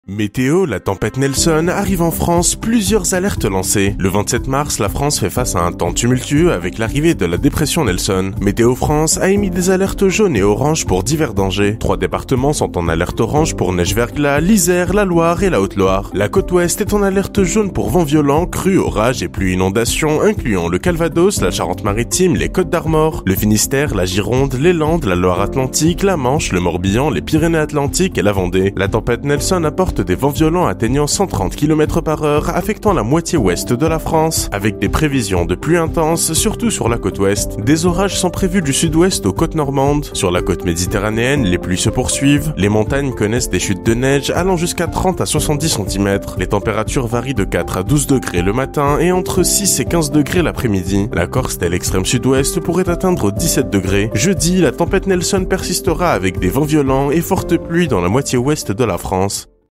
Météo : la tempête « Nelson » arrive en France, plusieurs alertes lancées !
C'est sous un temps fortement agité que se déroule ce milieu de semaine. L'arrivée d'une nouvelle dépression nommée « Nelson » n'arrange pas les choses. Vents violents, crues, neige-verglas... Météo France a émis de nombreuses alertes.